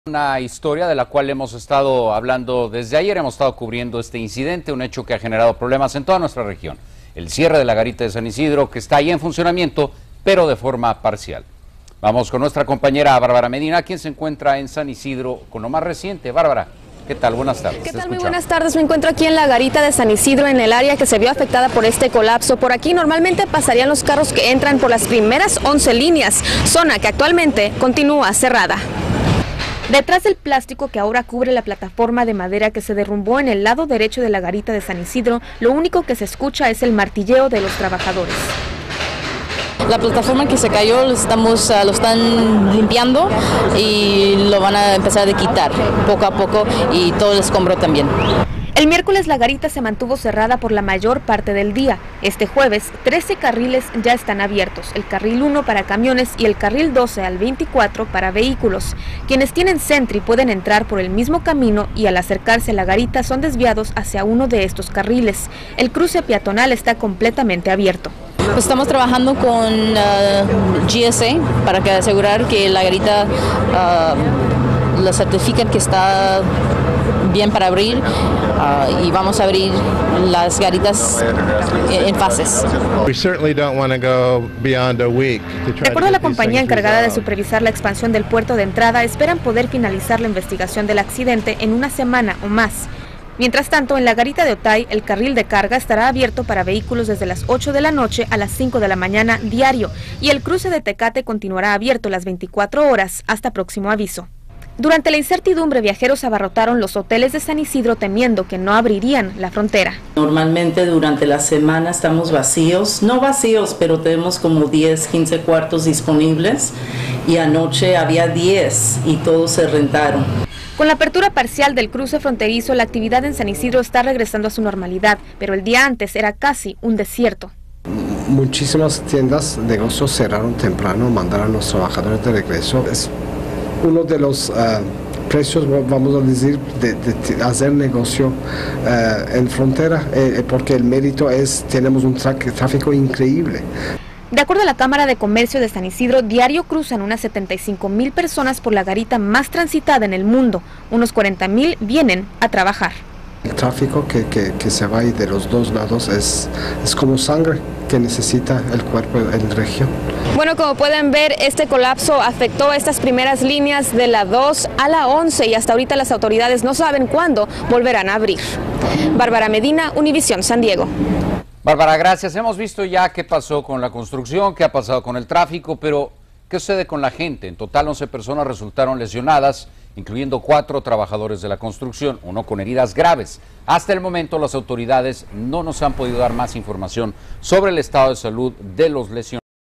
Abren parcialmente la garita de San Ysidro
San Diego. El cierre de la garita de San Ysidro ocasionó varios problemas de tráfico en Tijuana, sin embargo ya está en funcionamiento pero de forma parcial.
Los primeros 11 carriles en la zona que se derrumbó, actualmente continua cerrada. Detras del plástico que ahora cubre la plataforma de madera que se derrumbó en el lado derecho de la garita de San Ysidro lo único que se escucha es el martilleo de los trabajadores.